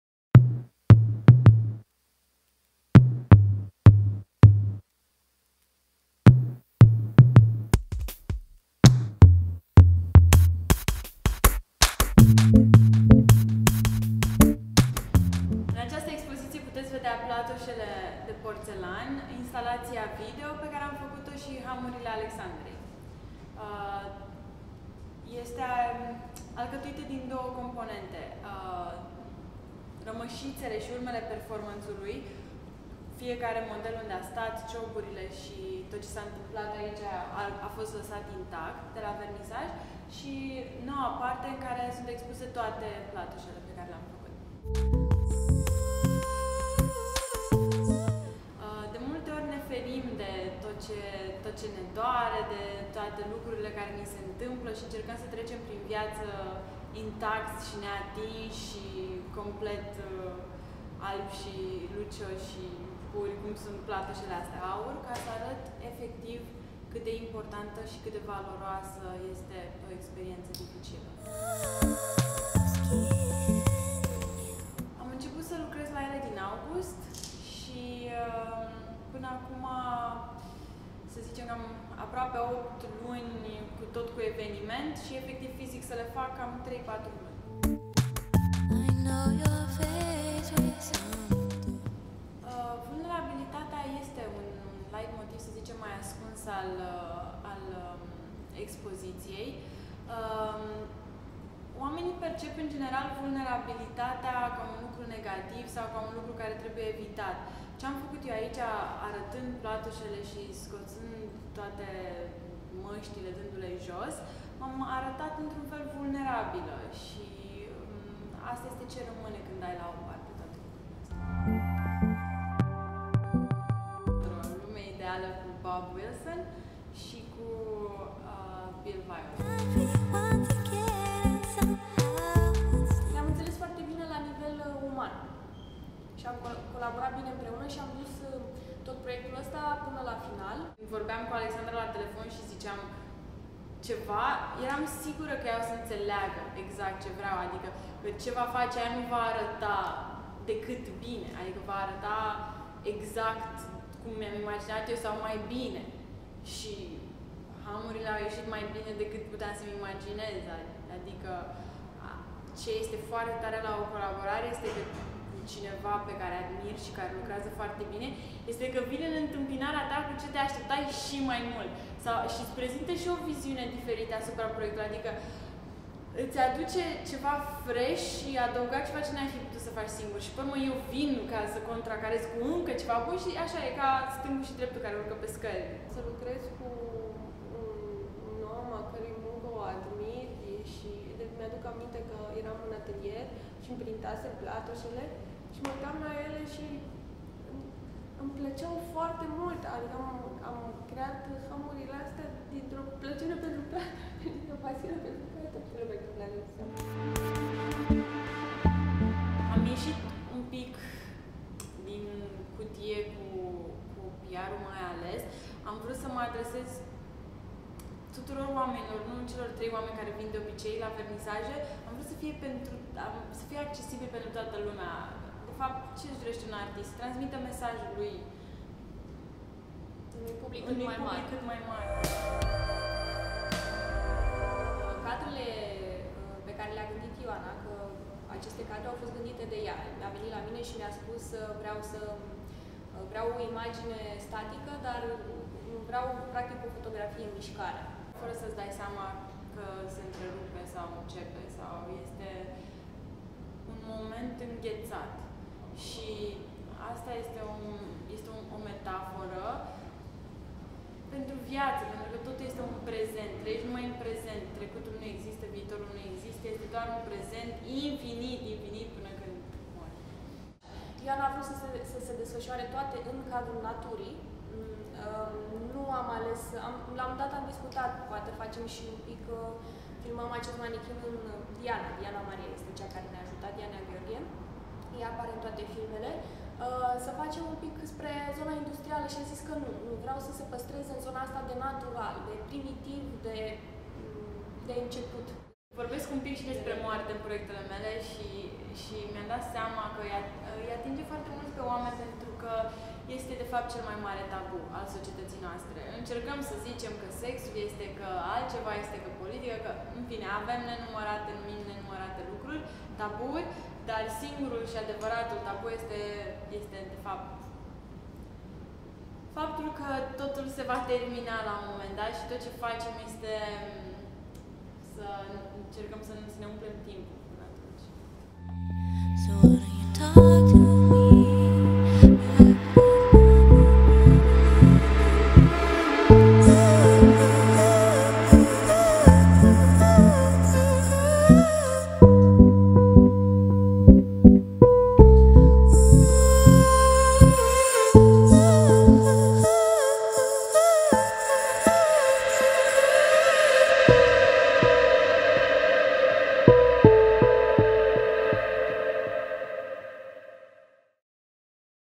Bleeding Gold | RO:DESIGN
A new video about 'Bleeding Gold' exhibition held in Bucharest  at Galateca Gallery . Ilina Schileru ,Ioana Sisea and Alexandra Abraham talk about what this project meant and how it has reached its final form.

Ioana Sisea presents the “Bleeding Gold “ project as a wearable armor made out of porcelain. The marks of experience transforms the integrity of the material, breaking the shields and making it bleed. But these bleeding is in gold, in ioana”s vision wounds have a way of crystalizing in a life altering experience: Every important happening leaves a mark on the human body. Visible or not, these marks add up to a topography of our personal history . Starting from a series of molds deriving from the female torso, a vulnerable and exposed part of the body, these pieces wear the mark of different wounds…”

Special thanks :
Galateca Art Gallery Ilina, Ioana and Alexandra.